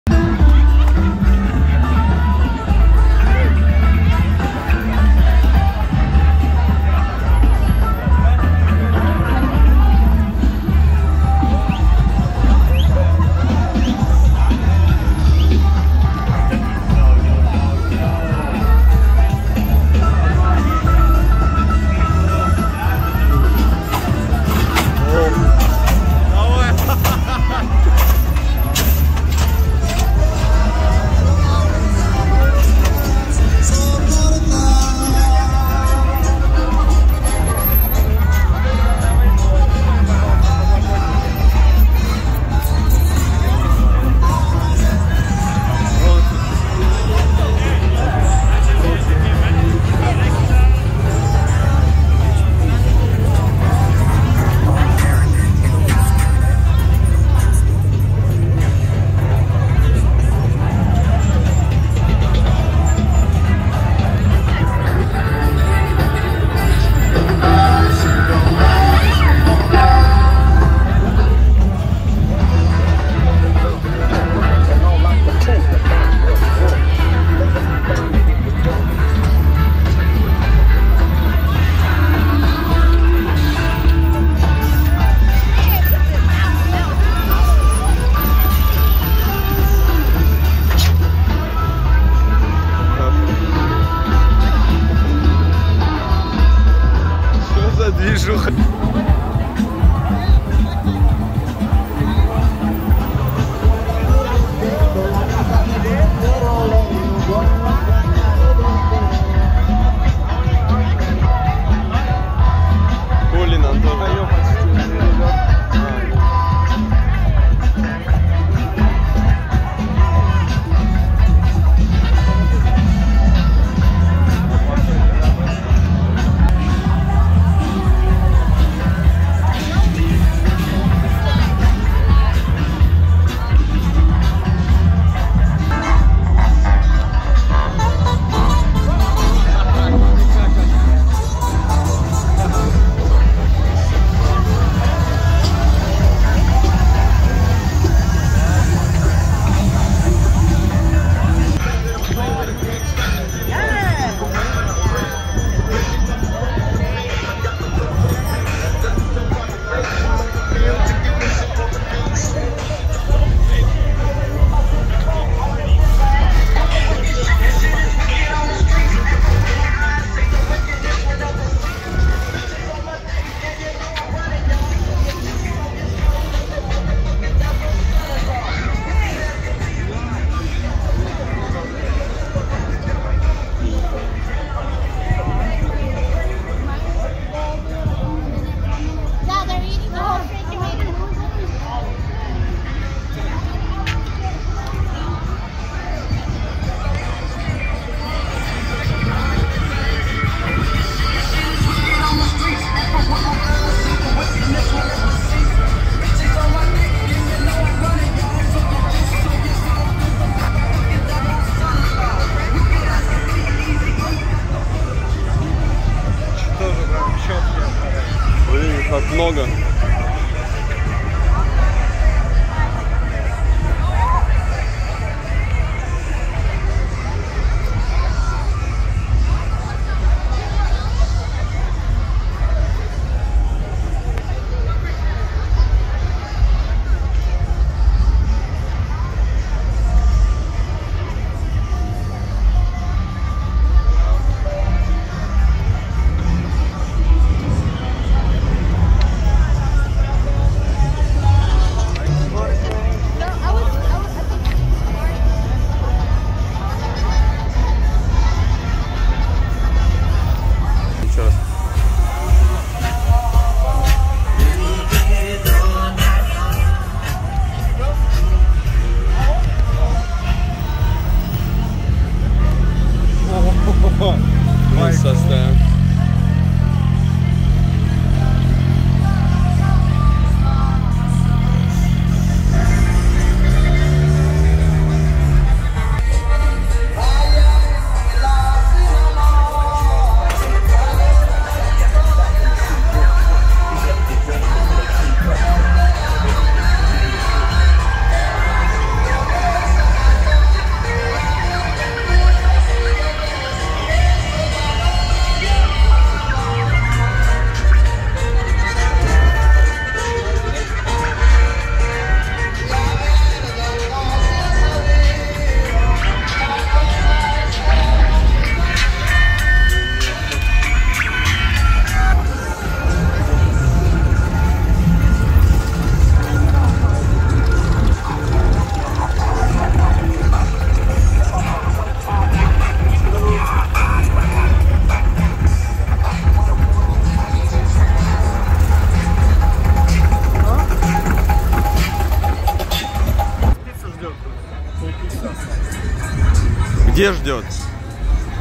Ретро автомобили, выставка в Америке
В Лос-Анджелесе проходил вот такой парад ретро автомобилей  ретро машины редкие автомобили